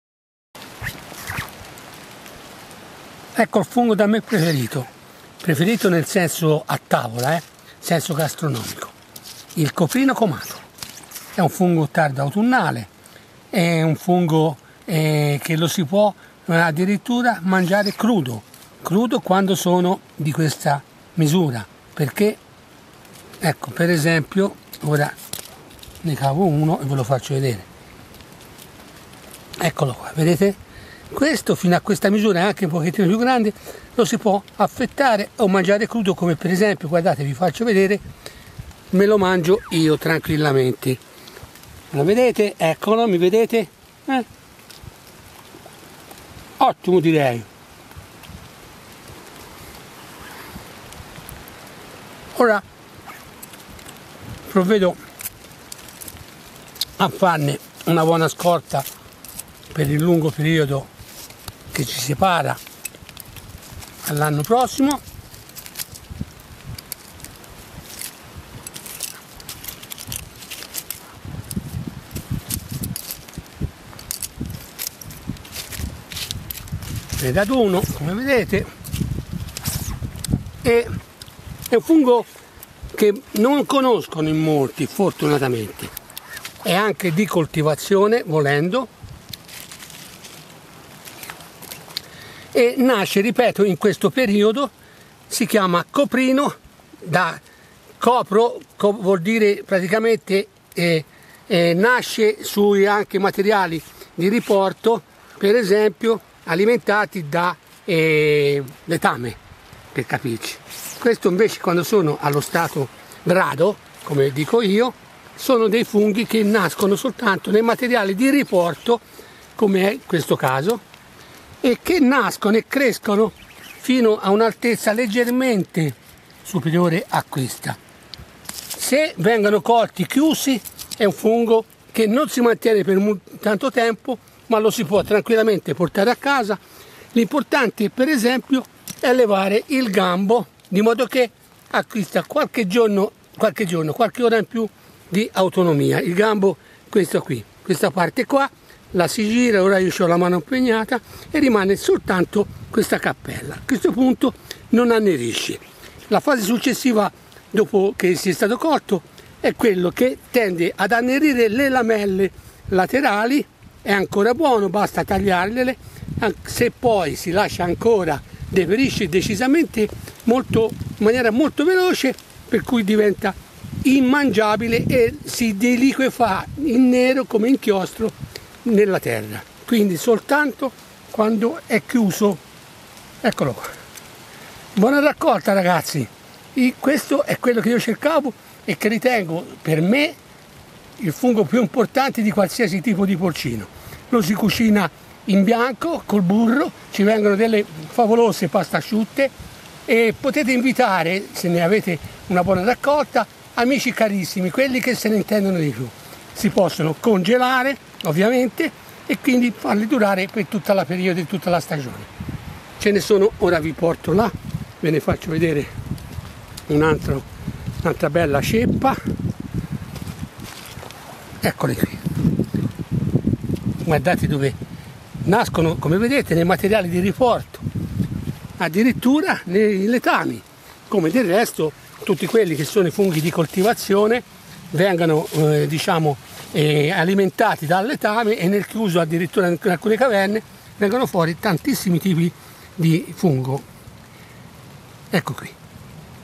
COPRINO COMATO ( Un FUNGO tra i più PREGIATI)
un meraviglioso mare di verde smeraldo il COPRINO COMATO è il fungo per eccellenza della cucina francese e non solo.